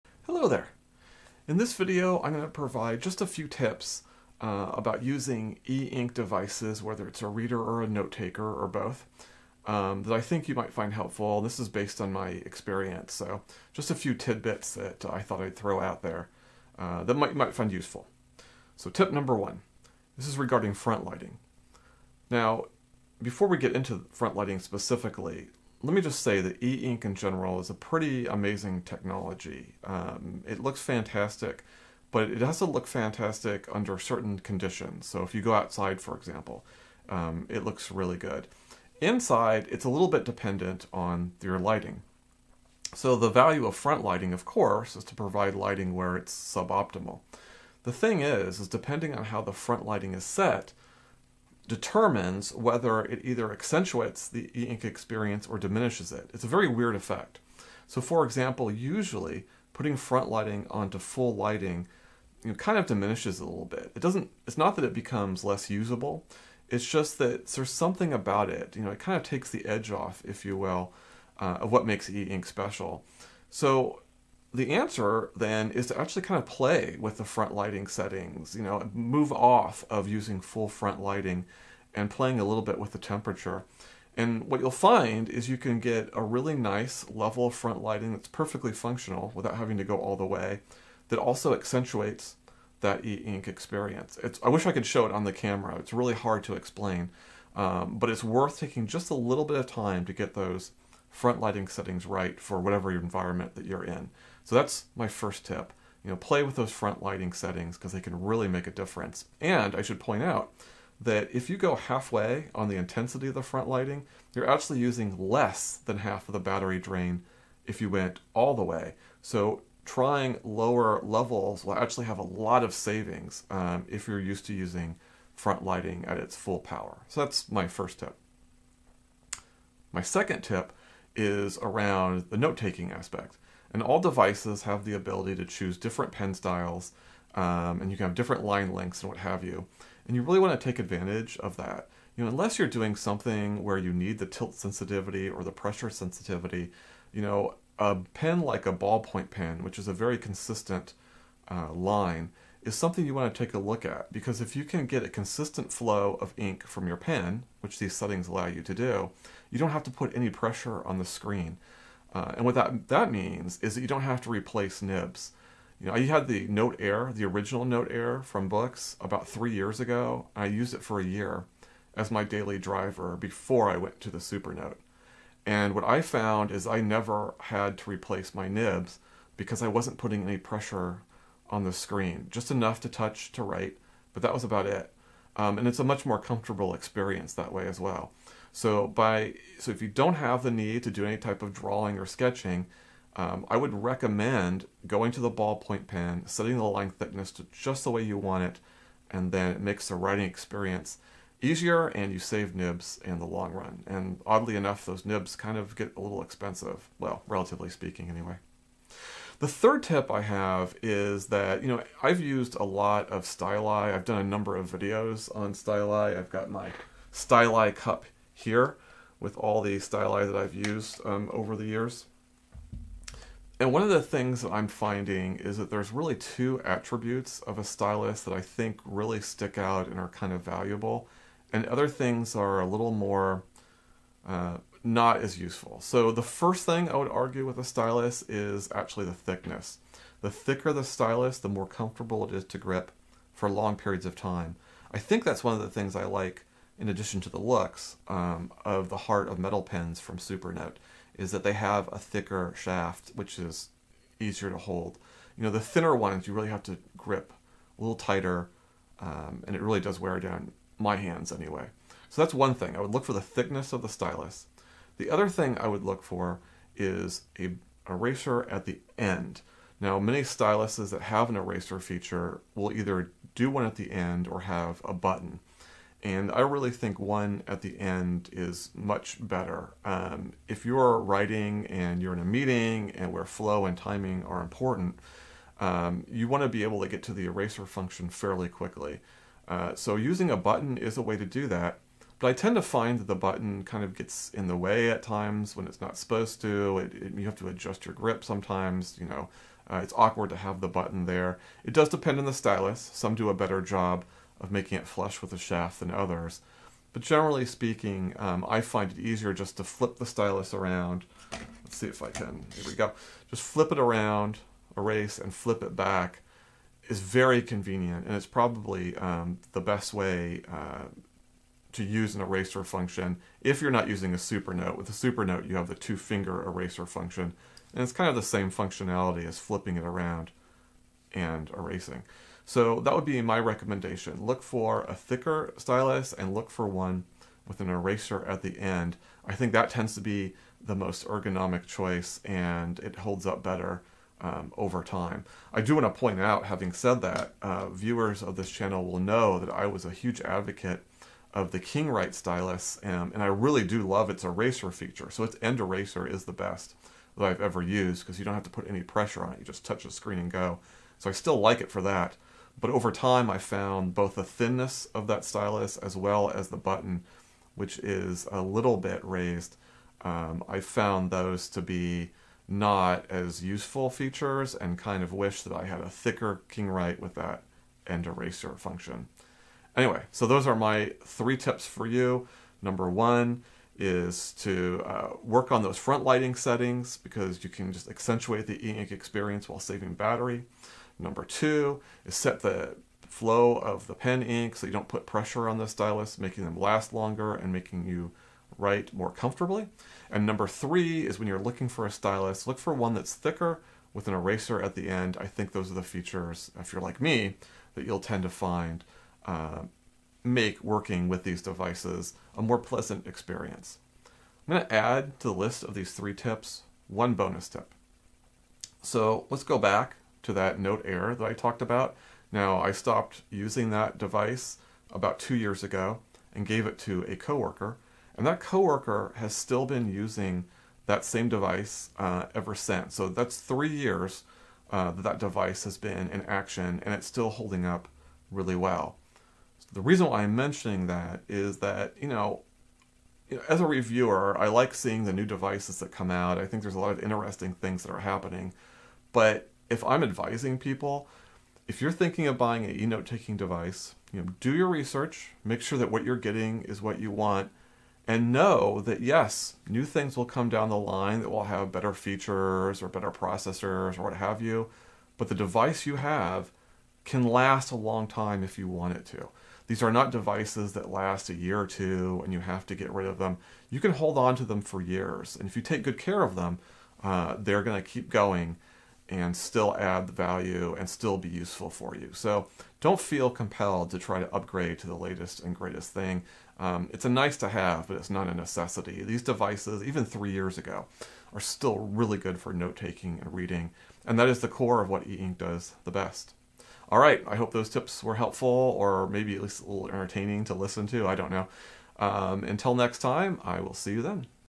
3 Tips for Using Eink Devices
Just thought I’d through a little wisdom out there, at least from one point of view.

0:00 Intro
0:19 Tip 1
2:10 Tip 2
3:30 Tip 3
6:56 Bonus Tip!